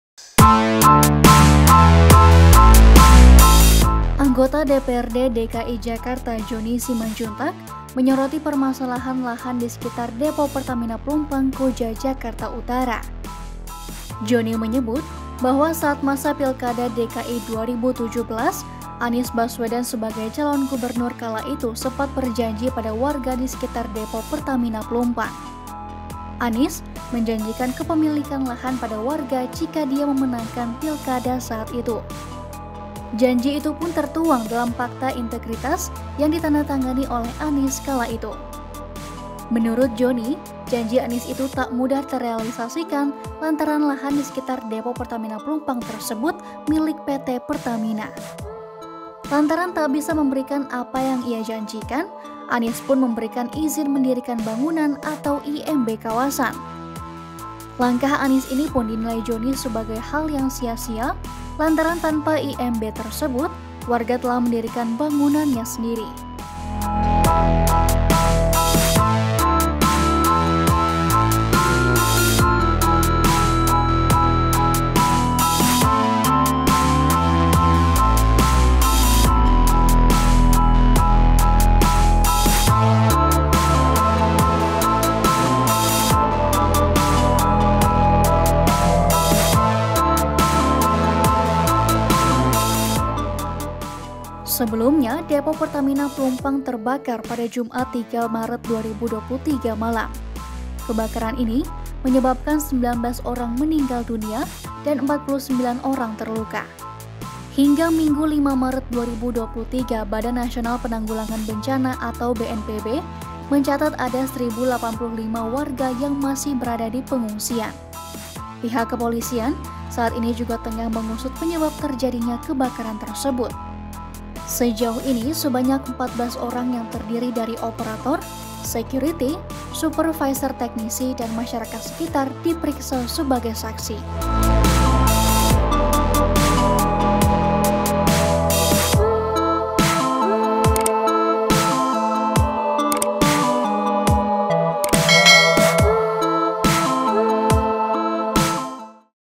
Anggota DPRD Soroti Janji Anies pada Warga Sekitar Depo Pertamina Plumpang
Anggota DPRD DKI Jakarta Jhonny Simanjuntak soroti permasalahan lahan di sekitar Depo Pertamina Plumpang, Koja, Jakarta Utara.

Jhonny menyebut bahwa saat masa Pilkada DKI 2017, Anies Baswedan sebagai calon gubernur kala itu sempat berjanji pada warga di sekitar Depo Pertamina Plumpang.

Anies menjanjikan kepemilikan lahan pada warga jika dia memenangkan Pilkada saat itu.
Janji itu pun tertuang dalam pakta integritas yang ditandatangani oleh Anies kala itu.

Namun menurut Jhonny, janji Anies itu tak mudah terealisasikan lantaran lahan di sekitar Depo Pertamina tersebut milik PT Pertamina.

Simak informasinya dalam video berikut.
Video Jurnalis: ARI, FIR, HAM
Penulis: Muhammad Naufal
Penulis Naskah: Anasthasya
Narator: Anasthasya
Video Editor: Alfiyan Oktora Atmajaya
Produser: Rose Komala Dewi